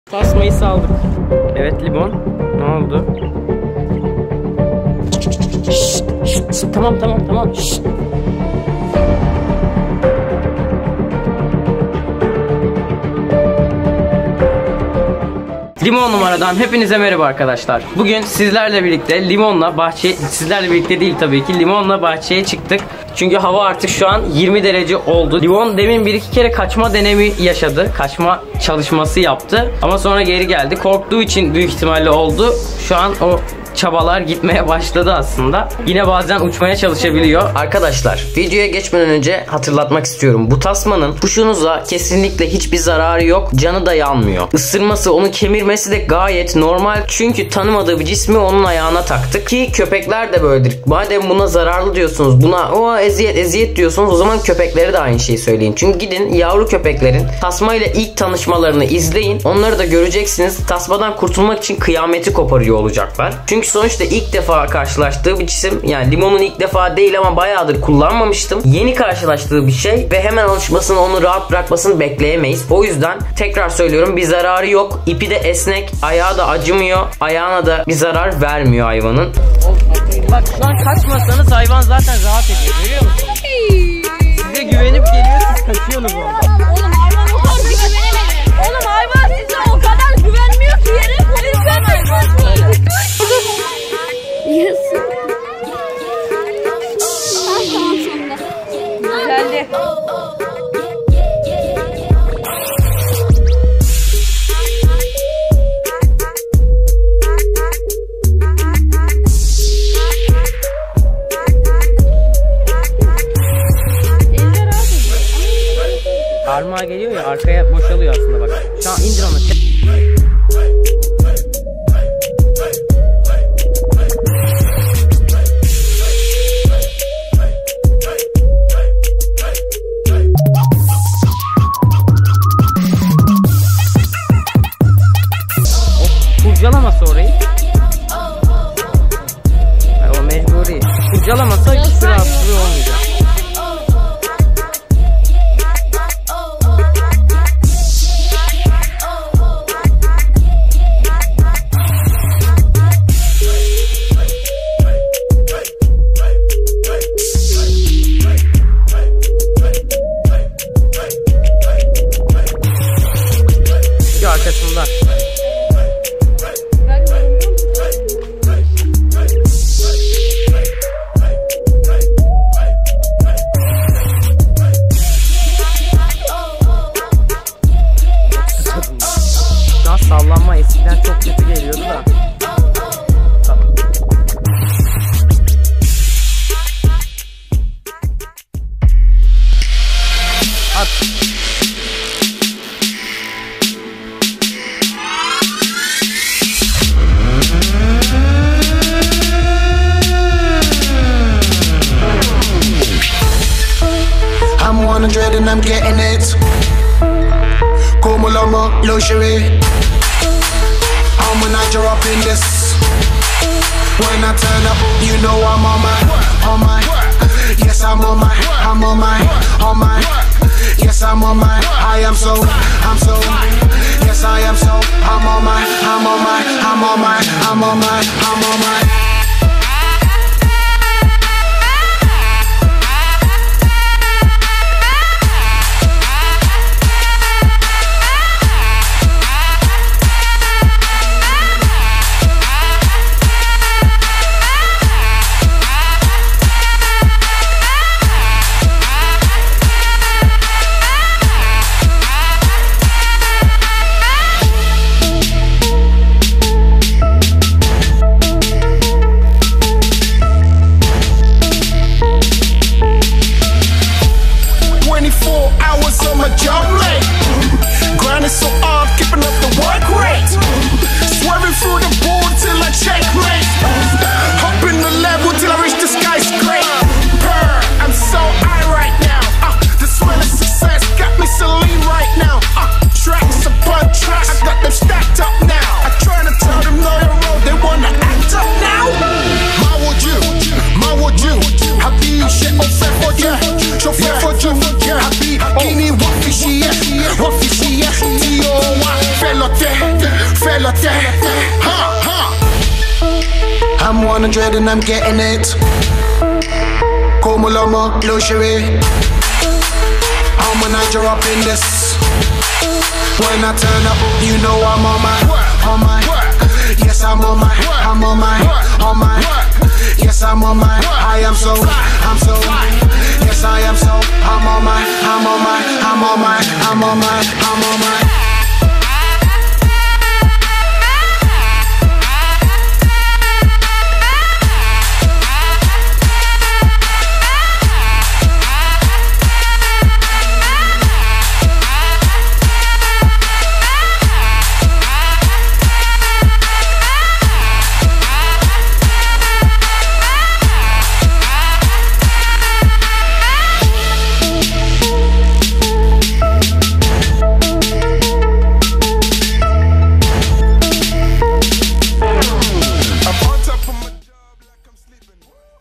Limonla Bahçeye Çıktım - Muhabbet Kuşu ile Sokağa Çıkmak - Kuş Sokağa Nasıl Çıkarılır ?
Merhaba arkadaşlar bu videoda sizin için limonla bahçeye çıktım. Burada muhabbet kuşu tasması yani muhabet kuşu bilek tasması kullandım. Muhabbet kuşu sokağa nasıl çıkarılır ki? Muhabbet kuşu bahçeye nasıl çıkar diye soranlara çok güzel bir tasmadır ayak tasması. Muhabbet kuşu bahçede yaşamaz o yüzden sakın orada bırakayın:P Limon = sokakta kaçmayan muhabbet kuşu.
-Beğeneceğiniz Diğer Videolarım-

-BÖLÜMLER-